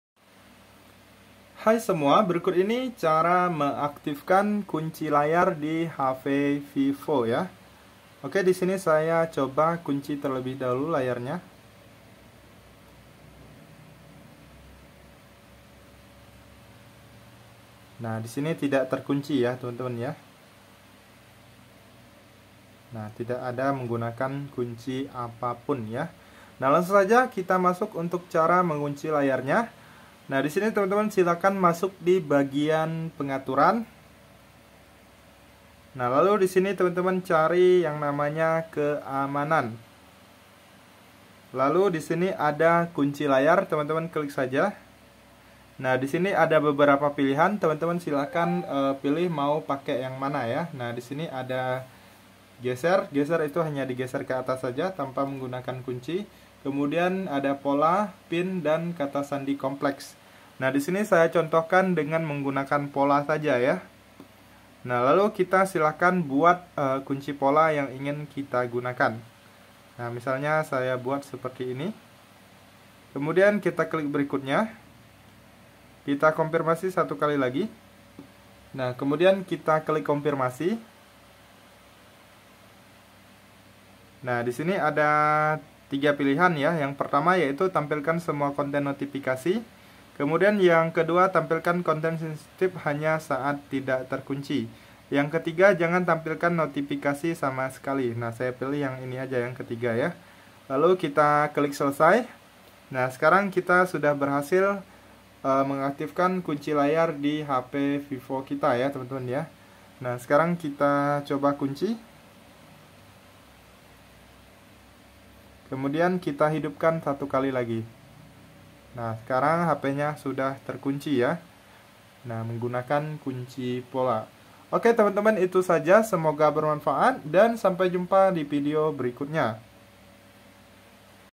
Cara mengaktifkan kunci layar hp Vivo V25e
Cara mengaktifkan kunci layar di hp Vivo V25e.

Berikut ini Cara mengaktifkan kunci layar di hp Vivo V25e.
Semoga bermanfaat 🙏